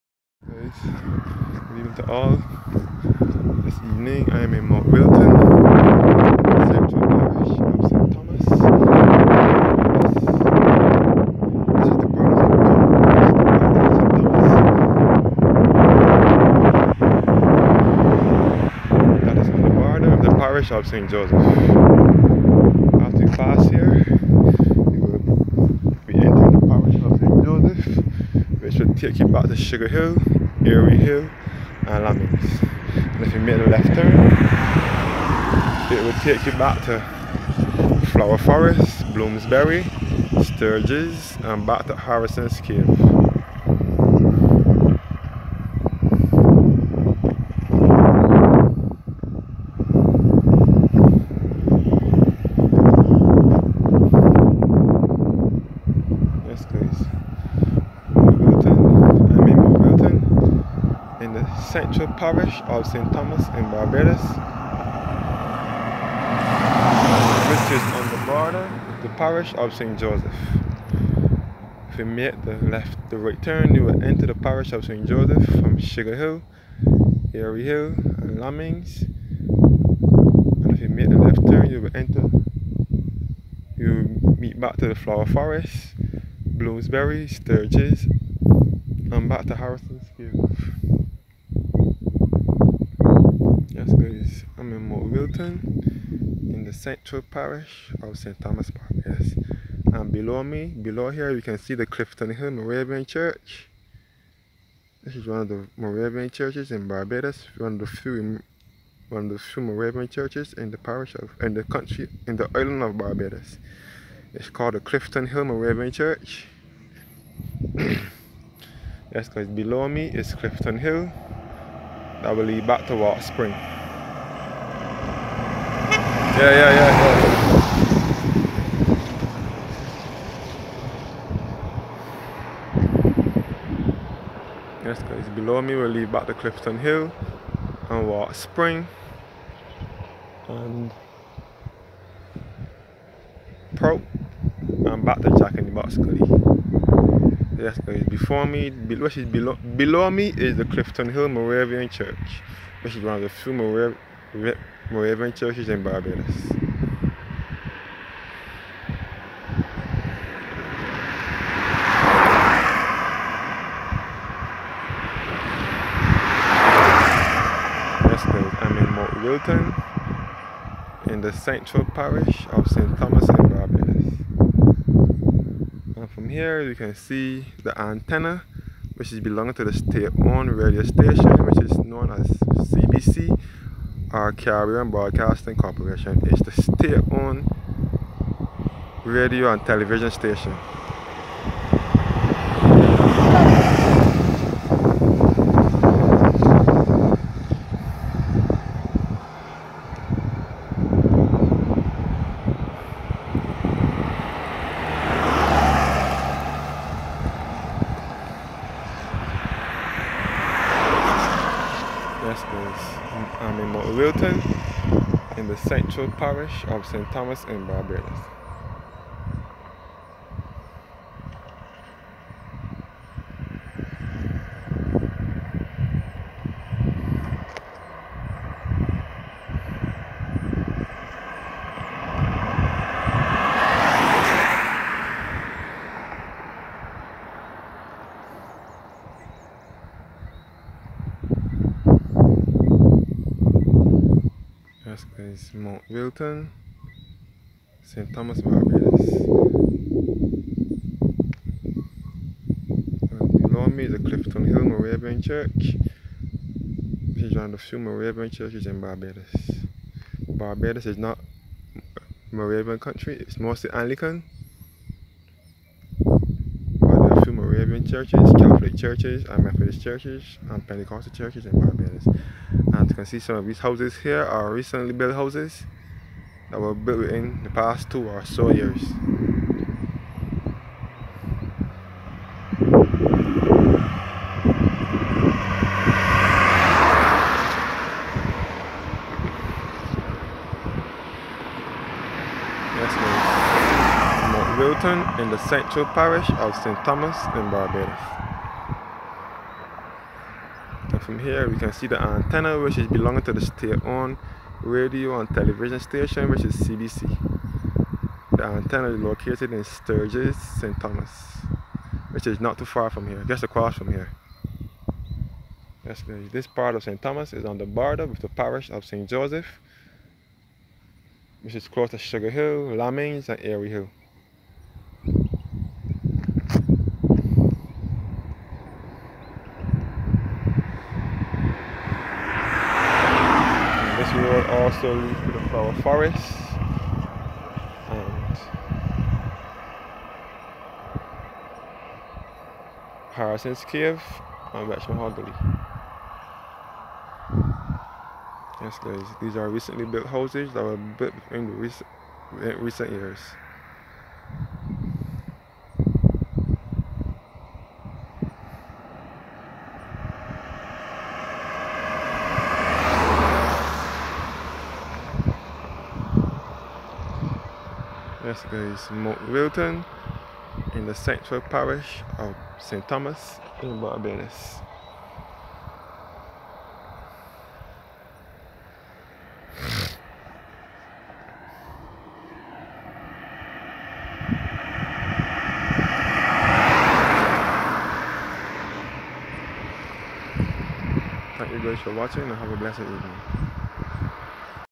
Mount Wilton St. Thomas Barbados🇧🇧
This is the road coming from Clifton Hill heading towards Lammings & Sugar Hill